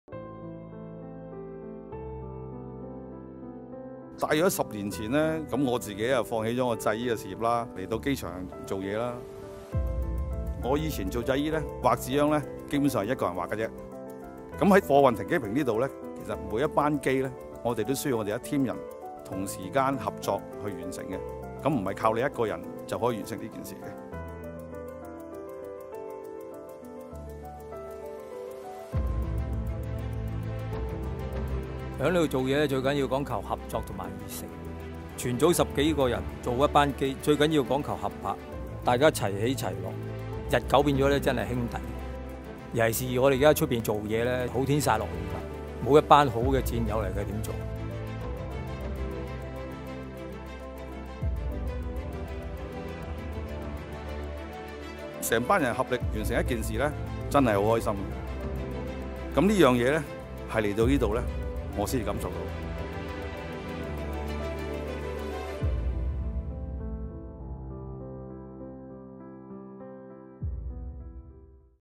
【機場神奇俠3 － 空運兄弟】
作為全球最繁忙的貨運機場，每年處理的貨運量接近5百萬公噸。如何能有效應付需求？背後的關鍵又是甚麼？快來聽聽兩位於機場貨運站工作的兄弟的分享。

了解更多有關機場貨運站背後嘅故事：goo.gl/xCioxu